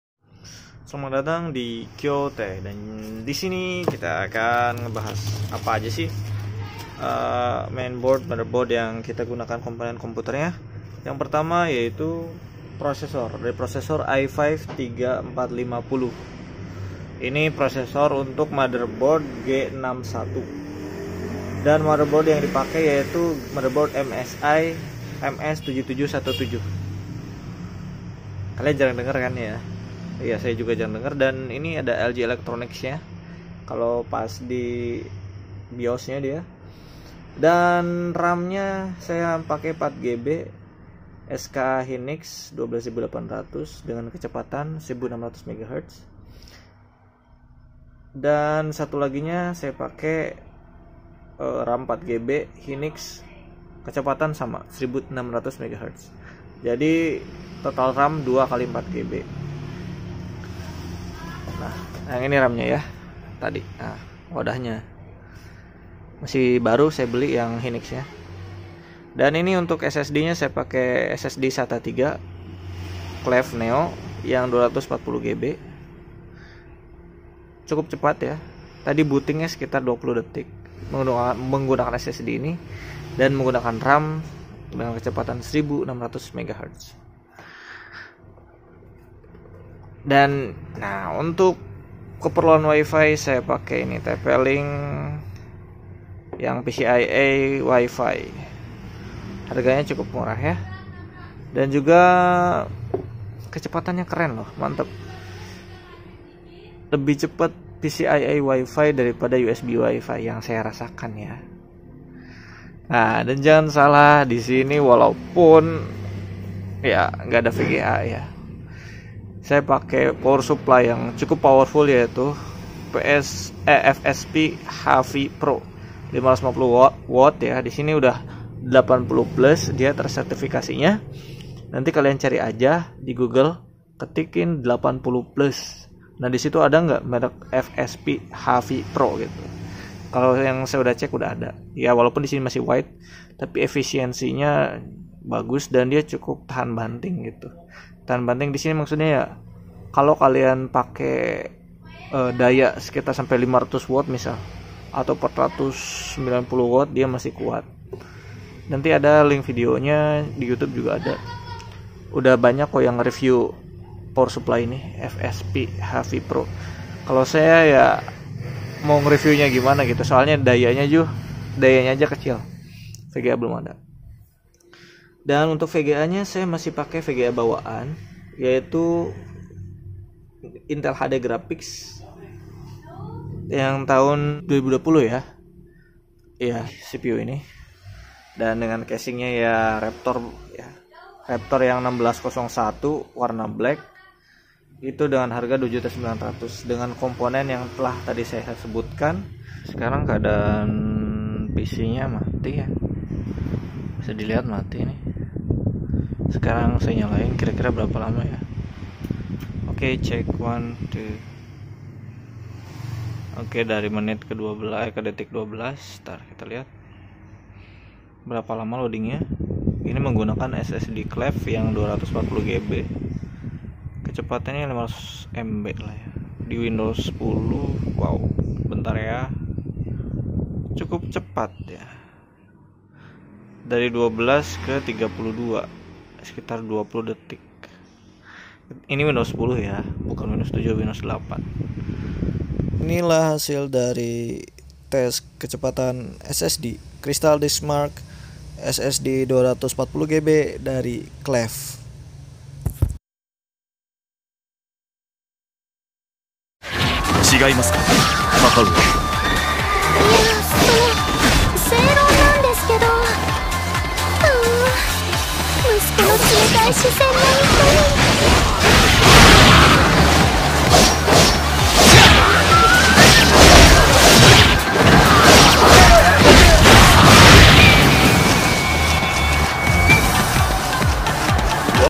PC PROSESOR I5 3450 VGA INTEL HD 2000 PSU FSP HV PRO 550W 80+ SSD 240GB KLEVV HARGA 2 JUTAAN
1. PROSESOR I5 3450 : "BEKAS".
2. MOTHERBOARD H61 MSI MS-7717 (LG ELECTRONICS) "BEKAS".
3. RAM 2X4GB DDR3 12800MHZ (SK HYNIX, HYNIX) "BARU".
4. PCIE WIFI 150MB/S TPLINK "BARU".
5. SSD SATA III 240GB KLEVV "BARU".
6. CASING RAPTOR 1601 BLACK (ADA LED RGB) "BEKAS RASA BARU".
7. PSU 550W 80+ WHITE FSP HSV (EFISIENSI TERJAMIN) "BARU".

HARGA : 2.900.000